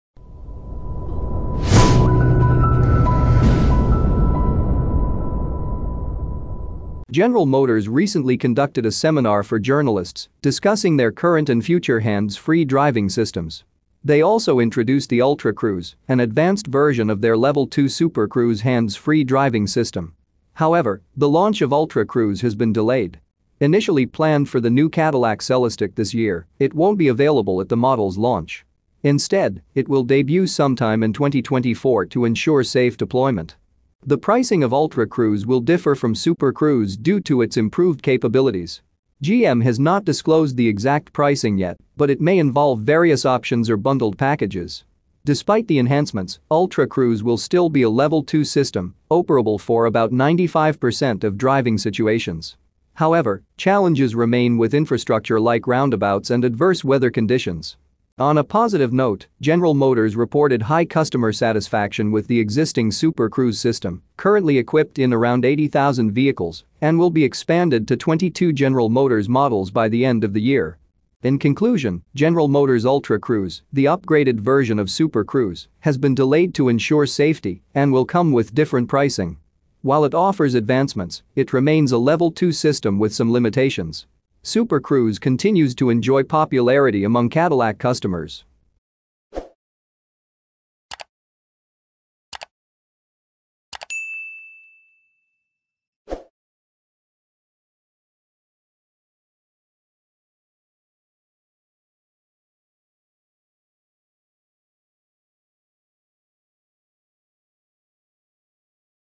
Discover the Future: General Motors ultra cruise delayed to 2024
In this video, we dive deep into the latest update from General Motors (GM) about their highly anticipated hands-free driving system, Ultra Cruise. Unfortunately, the news isn't what we expected - Ultra Cruise is delayed! Join us as we uncover the reasons behind the delay and what it means for the future of self-driving technology.

With Ultra Cruise now set to launch in 2024, GM is taking every precaution to ensure the system is safe and ready for the road. We explore the improvements that Ultra Cruise will bring compared to the current Super Cruise setup, making it a must-watch for all car enthusiasts and tech lovers.

Stay informed and be part of the driving revolution! 🚗💨